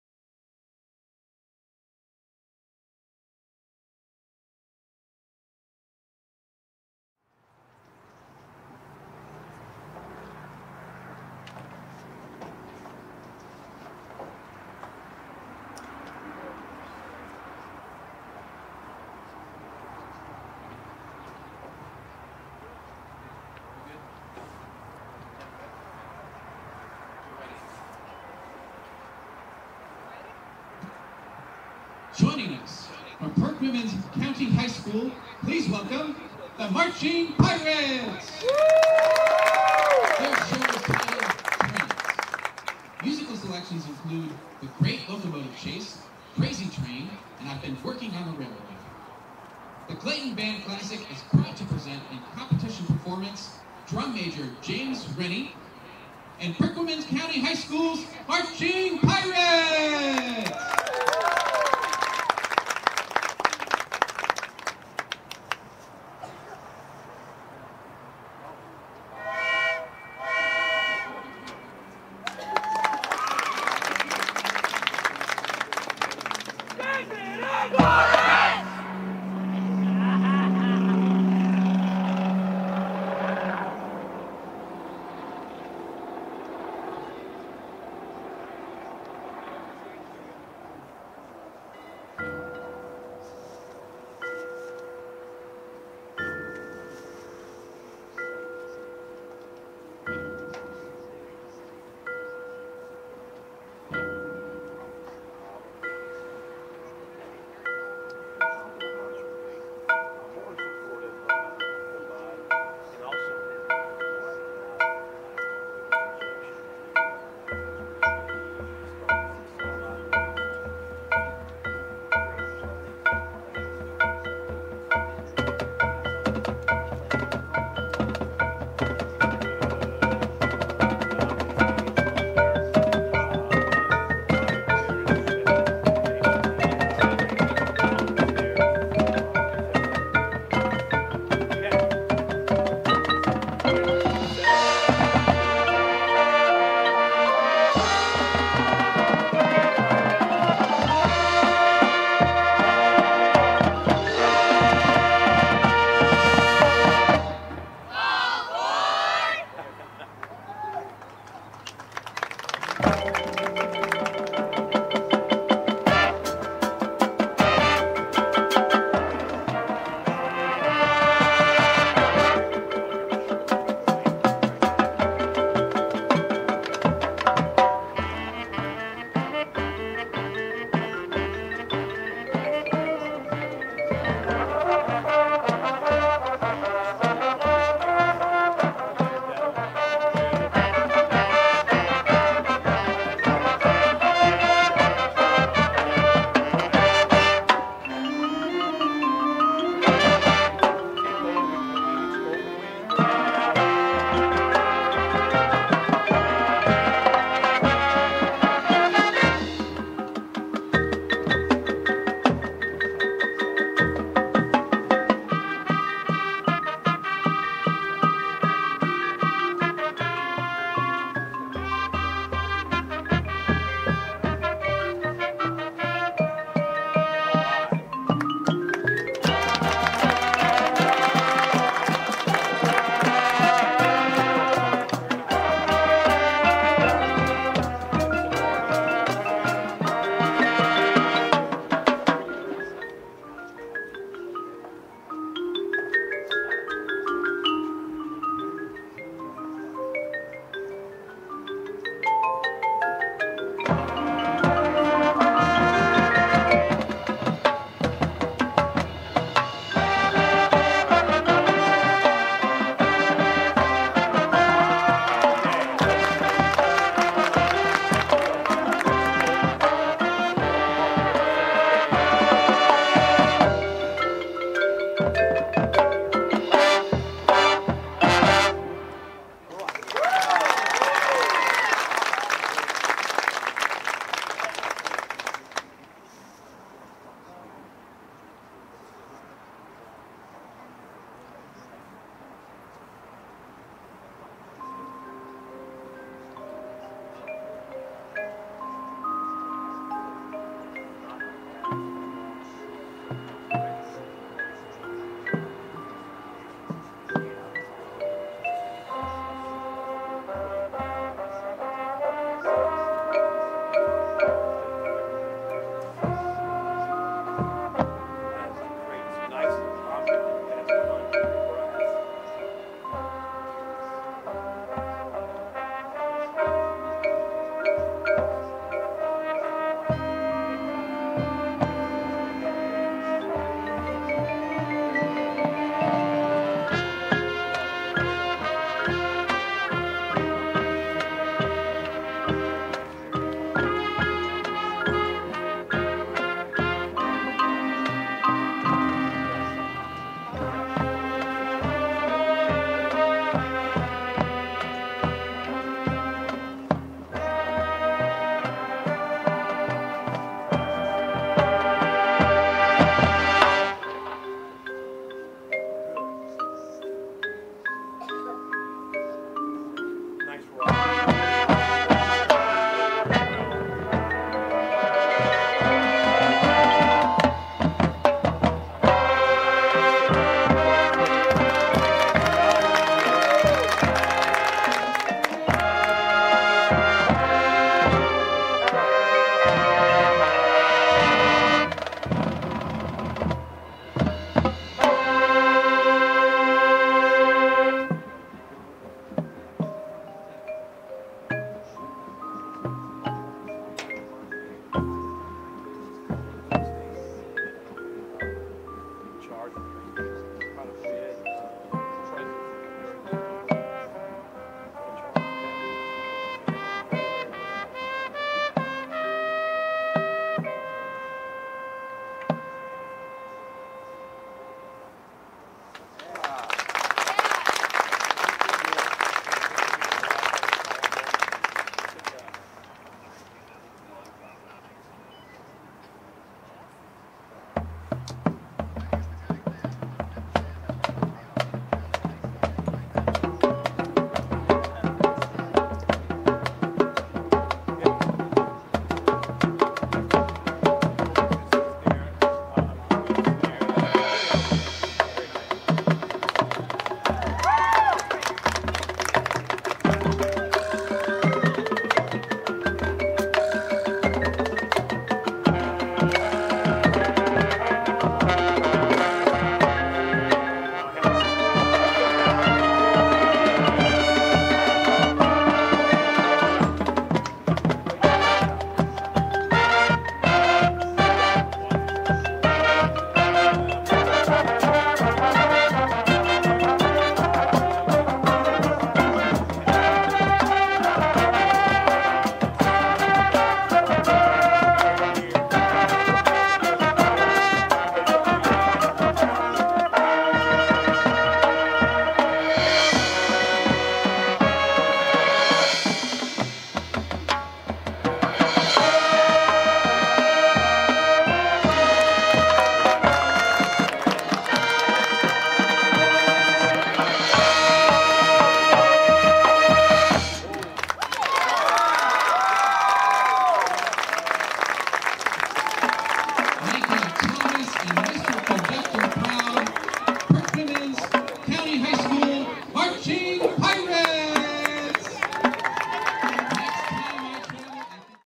Perquimans County High School Marching Band 10/21/2017
The 2017 Perquimans County High School (Hertford, NC) Marching Pirates perform their show at the 32nd Annual Clayton Band Classic hosted by the Clayton High School Marching Comets (Clayton, NC) on 10/21/2017.

The Pirates performed as a class A band.